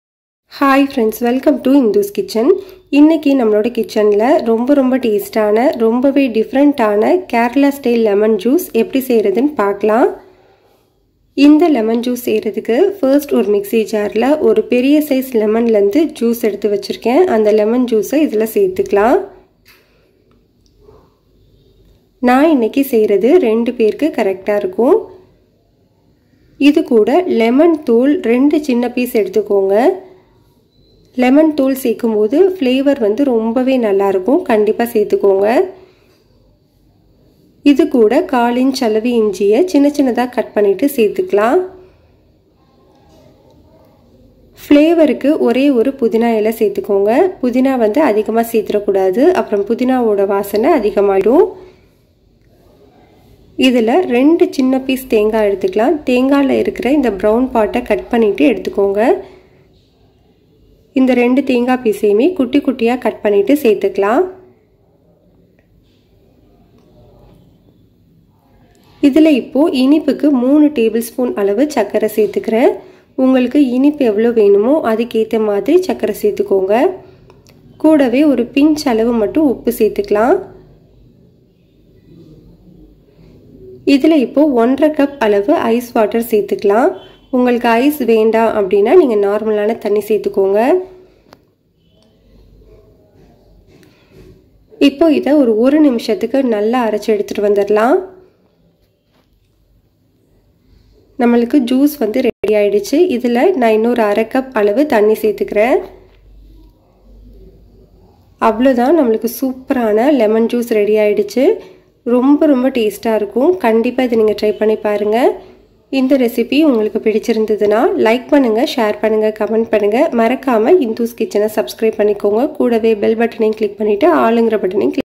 கேரளா லெமன் ஜூஸ் செய்வது எப்படி|Lemon juice in tamil|kerala lemon juice recipe in tamil
Ingredients

lemon -1
lemon skin - small piece
coconut - small piece
sugar - 3tbsp
salt - a pinch
water - 2cup